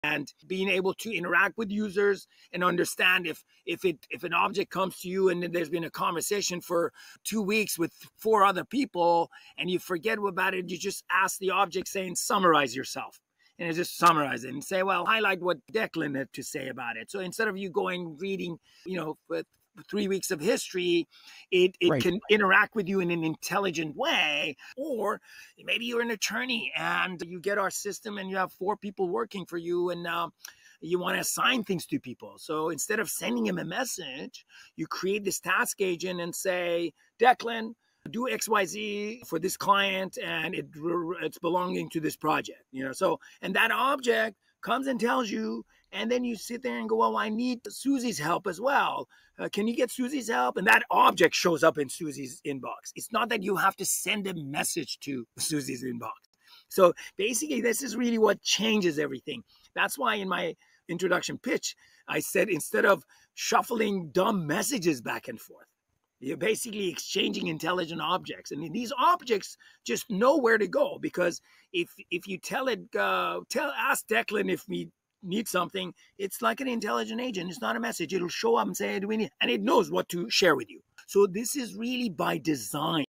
AI Agents move from basic to brilliant with Intelligent AI Objects
Picture this: instead of one big, all-knowing AI brain trying to run your entire business, imagine if every part of your operation had its own mini-AI agent.

Each document, each spreadsheet, each meeting - all with their own specialized intelligence. Sounds wild, right?

The Darwinian Leap: From Basic to Brilliant AI Agents - Lightning Fast Efficiency

- Who needs an LLM to help you make a pizza?

- AI Agent Evolution from Old Software Habits

- Moving into AI Agents - will it create chaos?

- Forget LLMs - Mini ChatGPTs Designed to Drive Business

- Waiting for AGI, for AI Cognition???

- Non technical people use AI Agents?

- Why is the AI industry taking the hard way?

- Organic AI copies models in nature